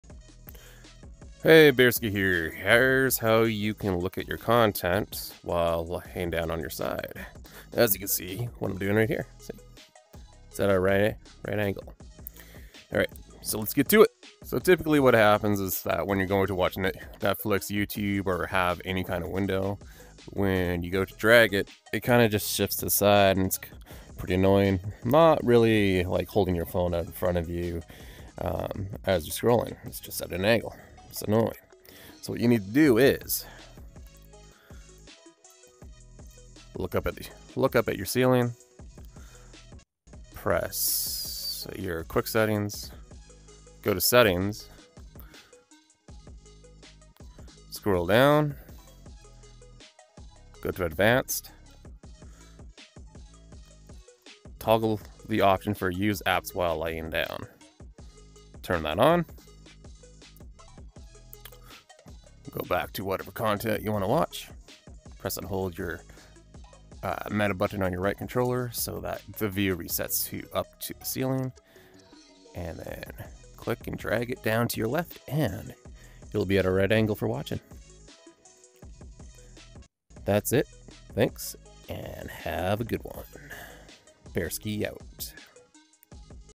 Rotate Quest 3 Windows When Laying Down Sideways!
Are you someone who loves relaxing with your Meta Quest 3 but wishes you could enjoy content while lying comfortably on your side? Whether you're winding down for the night or just want to switch up how you use your headset, this video is for you!

In today’s tutorial, I’ll walk you through exactly how to rotate your windows in the Meta Quest 3 so they appear sideways—perfect for when you’re lying on your side. With just a few quick settings tweaks and controller gestures, you’ll be able to view and interact with your apps from a whole new angle (literally!).

🔧 Here’s what we’ll cover step-by-step:

Navigating to the Advanced Settings menu inside your Quest 3 headset.

Enabling the "Use Apps While Lying Down" toggle to unlock rotated window support.

Positioning yourself—just lie down on your side and look straight up at the ceiling.

Using the Meta button (located on your right controller) to grab and drag your window into the ideal position.

Adjusting the window placement for the most comfortable side-viewing experience possible.

Kicking back, relaxing, and enjoying your favorite content hands-free and neck-strain-free.

This trick is a total game changer for side sleepers, loungers, and anyone who wants to make their VR time even more comfy. No more awkward angles or straining to keep your headset aligned while lying down!

💡 Pro Tip: This feature works great for streaming apps, browsing the web, or even just navigating menus while resting.

Have questions or other comfort hacks for the Quest? Drop them in the comments—I’d love to hear how you're using your headset in creative ways.

Let me know if you want to make it even more casual, add timestamps, or tailor it to a specific type of content viewer!